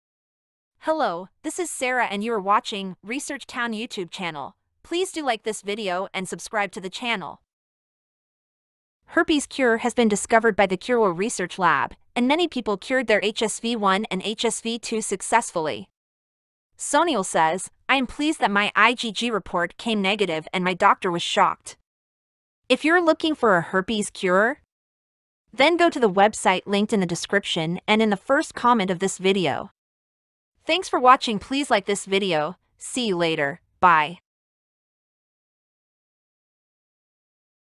How To Cure Herpes (HSV-1 HSV-2) | Research Town 🌳
In this video, I'm sharing with you the amazing story of how i discovered a herpes cure that works,